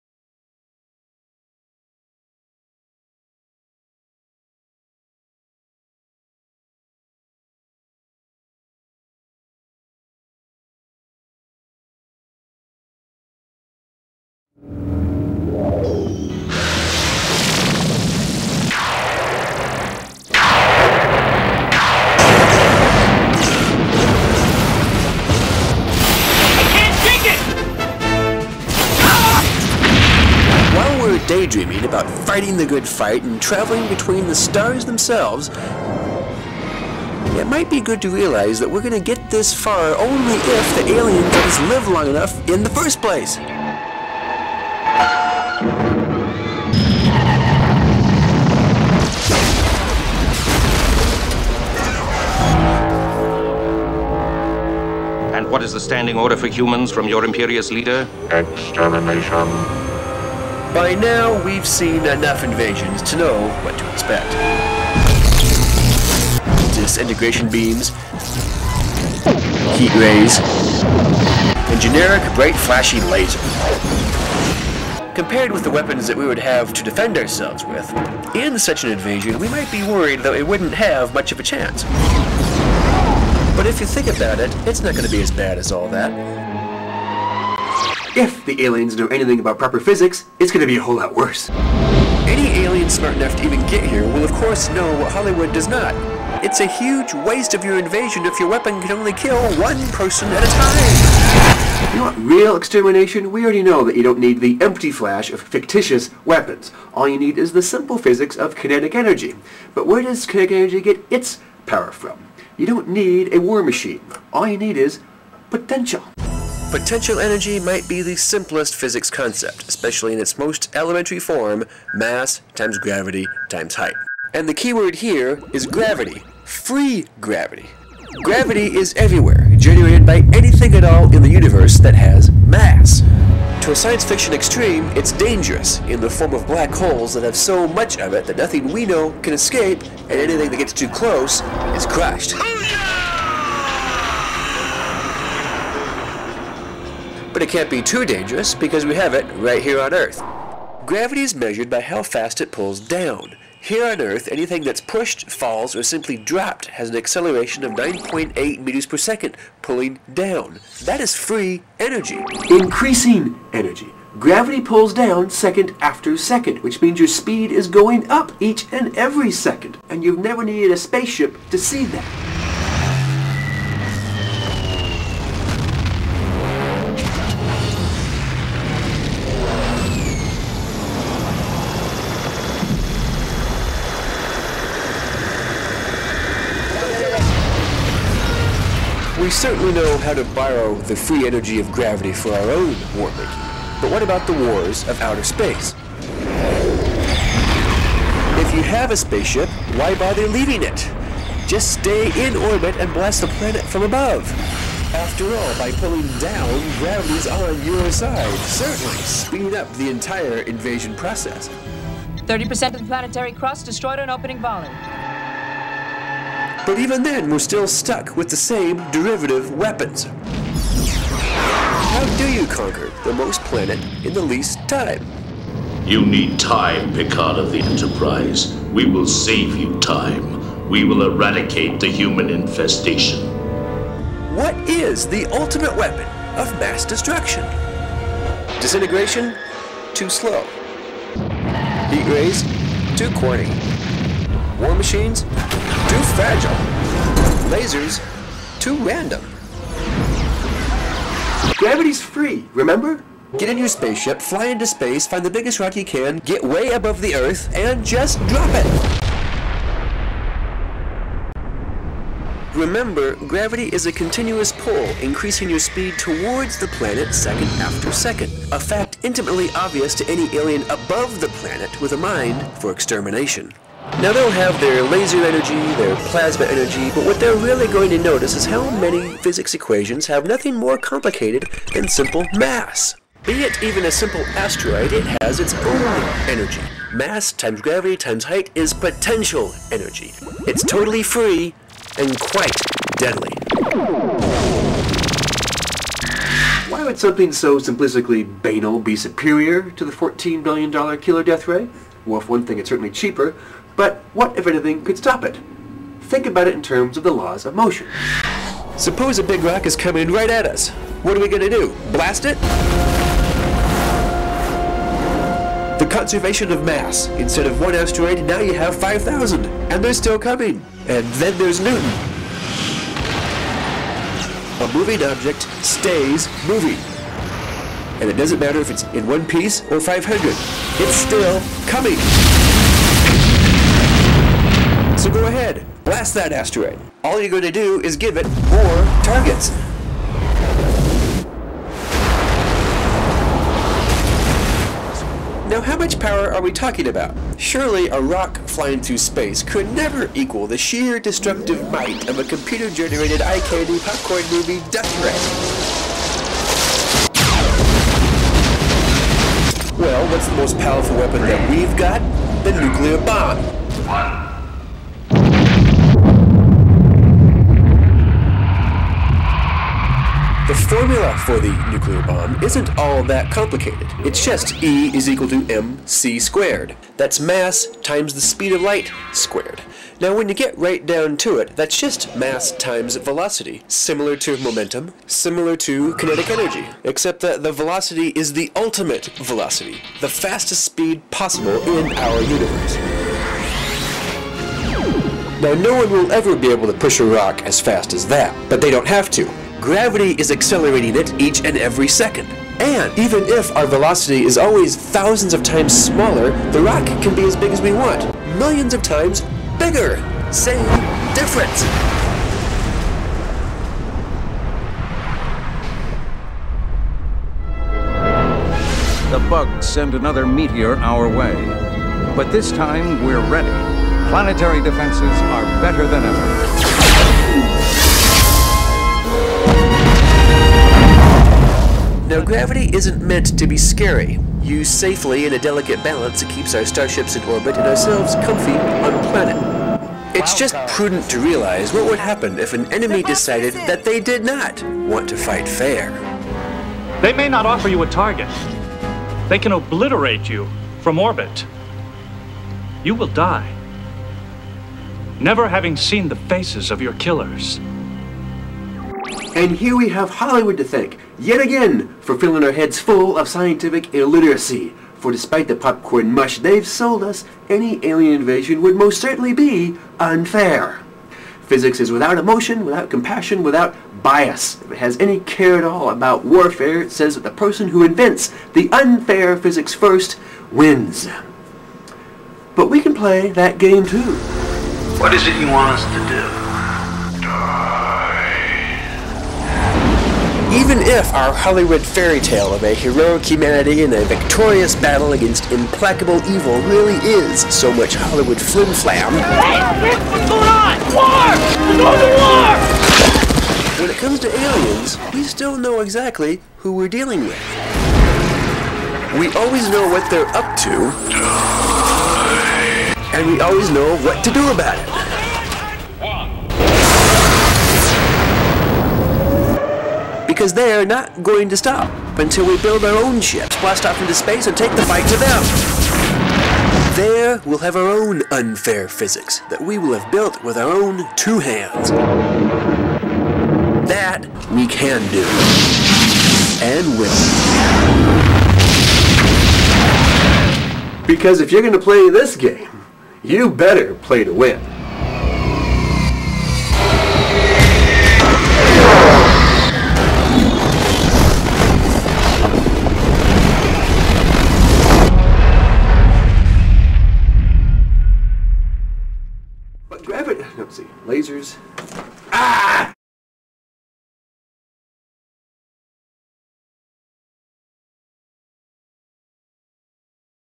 The Physics of Starship Battles: Gravity and Potential Energy UPGRADED SPREADINGTHEMUSE Productions
Remember all the times I said I was going to reupload my previous movies at higher quality with fixed sound?  Well here we are.  Only took me a year to get around to it. ;)
By the time I'm done 6 movies total will be reuploaded, at 10x the previous quality and with that annoying left/right speaker problem wiped out and done.   Enjoy